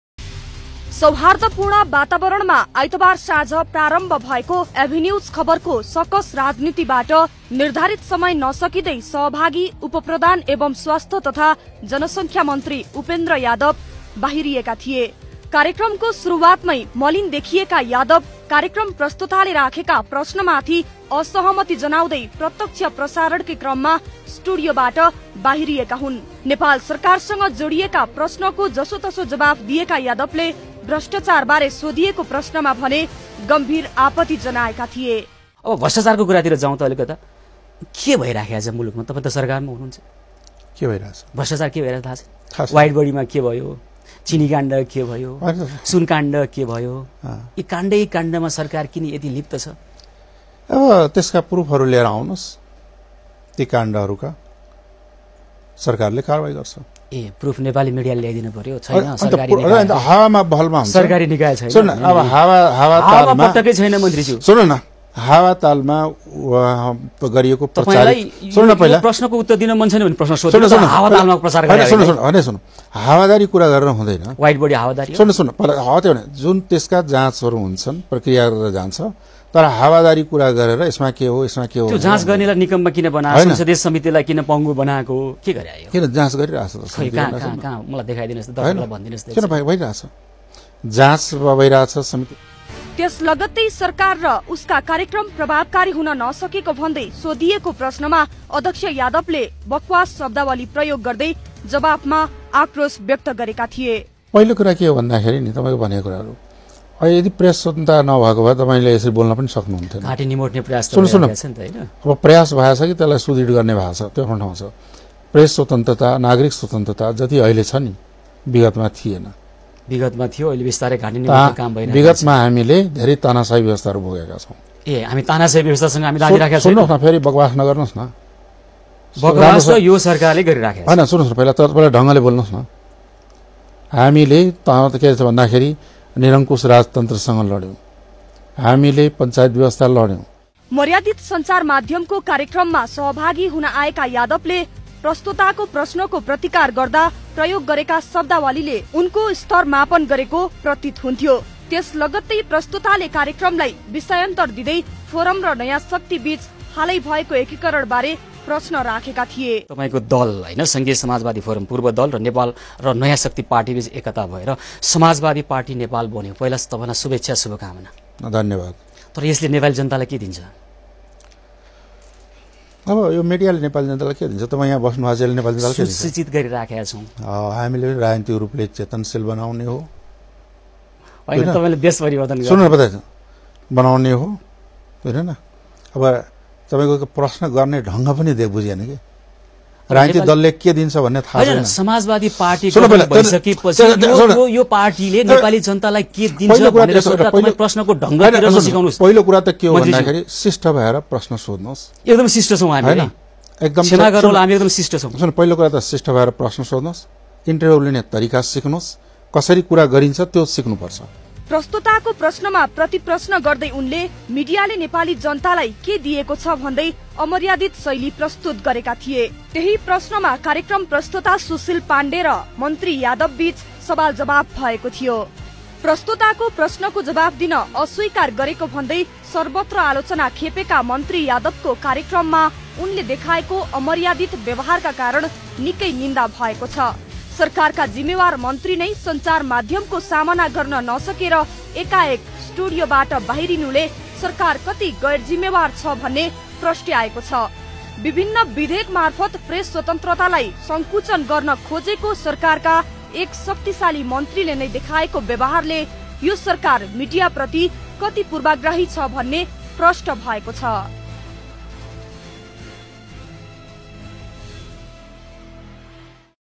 birgunjsanjal.com ,av news birgunj सरकार र नागरिकबीचको दुरी बढी रहेका बेला उपप्रधान एवं स्वास्थ्य तथ
सरकार र नागरिकबीचको दुरी बढी रहेका बेला उपप्रधान एवं स्वास्थ्य तथा जनसंख्या मन्त्री उपेन्द्र यादबले दुनियाँभरका दर्शकसमक्ष अलोकतान्त्रिक व्यवहार देखाएका छन् । एभिन्यूज खबरको कार्यक्रम सकस राजनीतिमा प्रश्तोताको प्रश्नको जवाफ दिन अस्वीकार गर्दै स्टुडियोबाट बाहिरिएपछि सरकारको सञ्चार माध्यमप्रतिको नियत झनै उदांगिएको छ ।